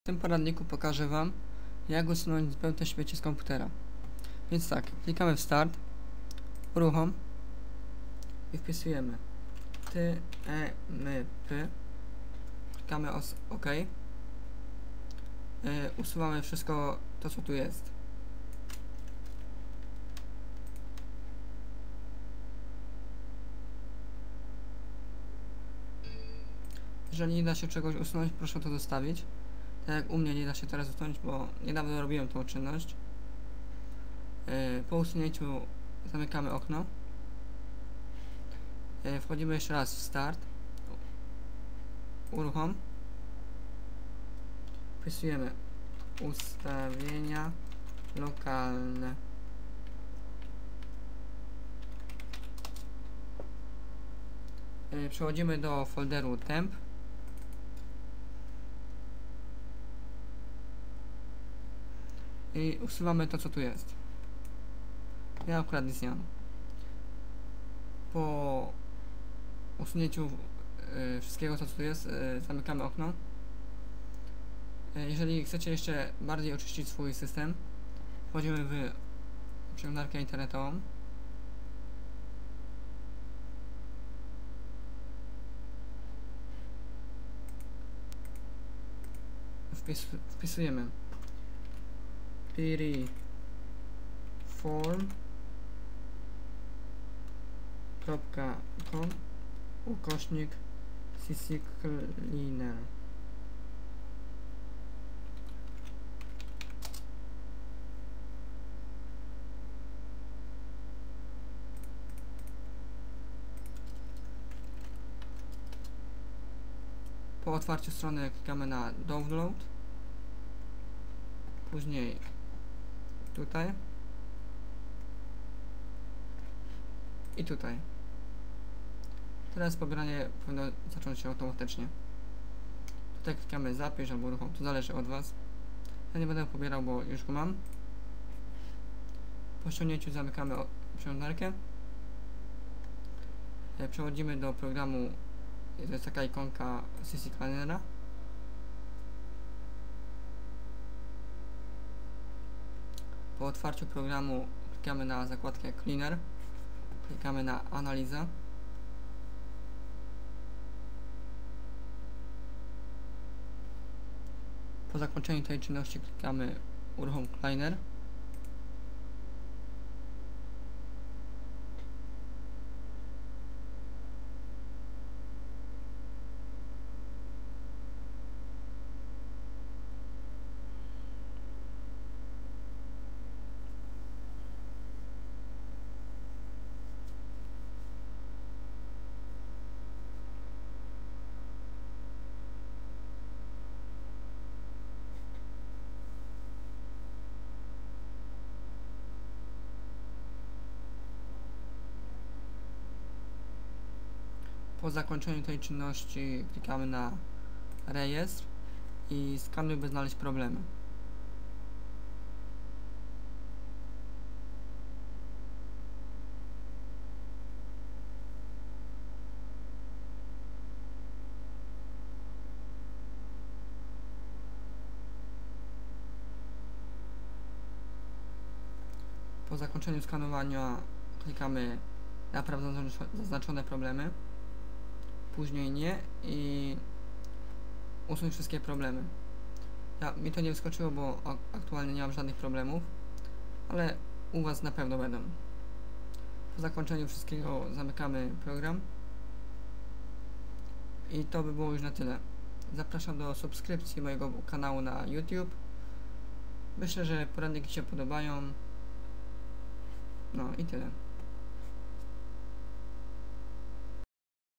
Jak usunąć zbędne śmieci z komputera?
Cześć w Tym poradniku pokażę wam jak usunąć zbędne śmieci z komputera.

Potrzebne będą :

CCleaner